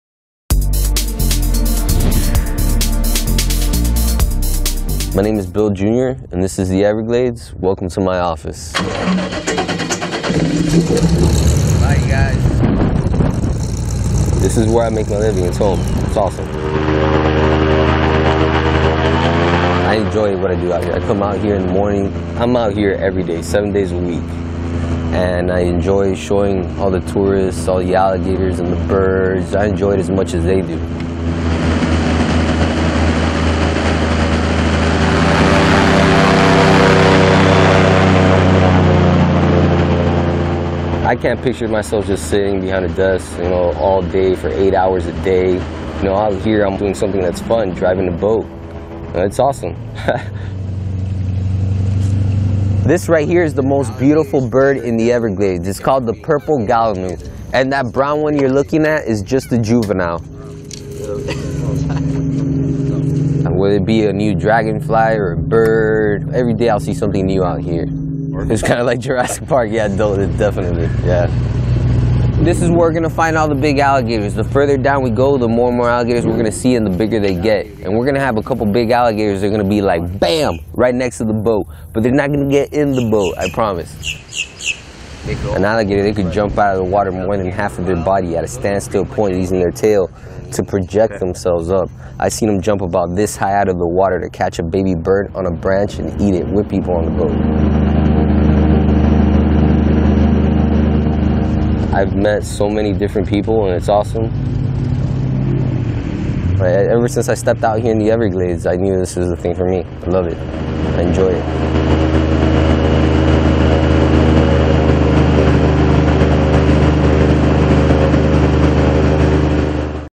Voices of America: Bill Alvarez, Everglades Guide
(Click the cc button to watch with captions)

My name is Bill Junior, and this is the Everglades. Welcome to my office.

This is where I make a living. It's home. It's awesome. I enjoy what I do out here. I come out here in the mornings. I'm out here every day -- seven days a week. And, I enjoy showing the tourists all of the alligators and the birds. I enjoy it as much as they do.

I can't picture myself just sitting behind a desk, all day, for eight hours a day. Out here, I'm doing something that's fun -- driving a boat. It's awesome!

This right here is the most beautiful bird in the Everglades. It's called the purple gallinule. And that brown one you're looking at is just a juvenile. Whether it be a new dragonfly, or a bird, every day I see something new out here. It's kind of like Jurrasic Park! Yeah, definitely.

This is where we will find all of the big alligators. The farther down that we go, the more and more alligators we'll see -- and the bigger they get. And, we're going to have a couple of big alligators, and they're going to be, like, "bam!" right next to the boat. But they're not going to get in the boat. I promise!

An alligator - they can jump out of the water more than half of their body [length] at a standstill point by using their tail to project themselves up. I've seen them jump about this high out of the water to catch a baby bird on a branch and eat it - with people on the boat.

I've met so many different people - and it's awesome! Ever since I stepped out here in the Everglades, I knew this was for me. I love it! I enjoy it.